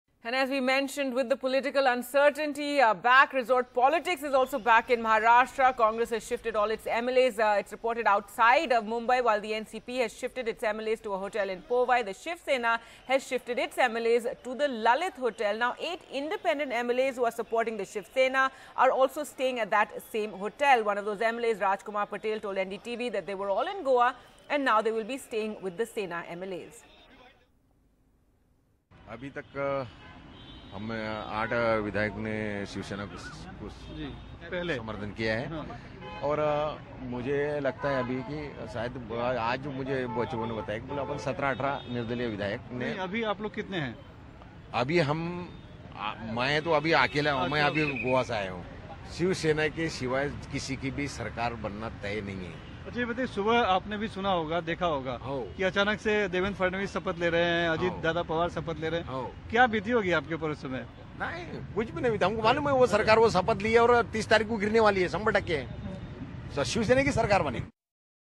Congress, NCP, Sena Move MLAs To Mumbai Hotels To Keep Flock Together
Maharashtra Government 2019: Swift political developments in Maharashtra - with the stunning return of the BJP to power in the state - have prompted the Shiv Sena, Sharad Pawar's party and the Congress to move their MLAs to different hotels. The MLAs have been sequestered in an attempt to protect them from poaching.